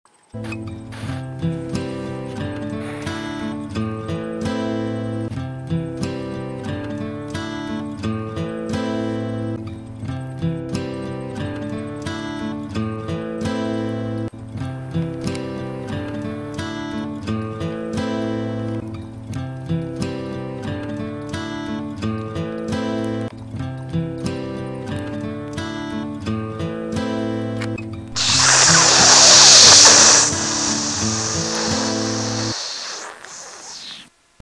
Burning Desire
We all love toasting marshmellows but egg loves pole vaulting into the fire